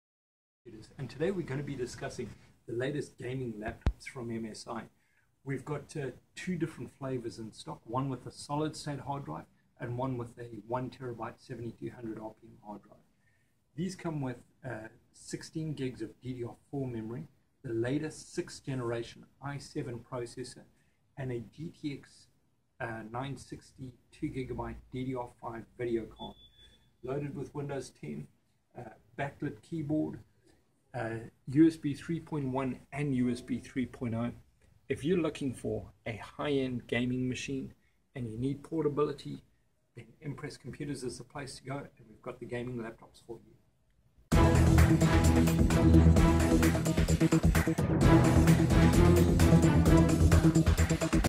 MSI Gaming Laptops IN Katy TX and Houston Texas from Impress GTX960 i7 128GB SSD and 1TB USB3.1
impress Computers
21733 Provincial Blvd
Ste 110

Virus Removal
Networking
Network Support
Managed Services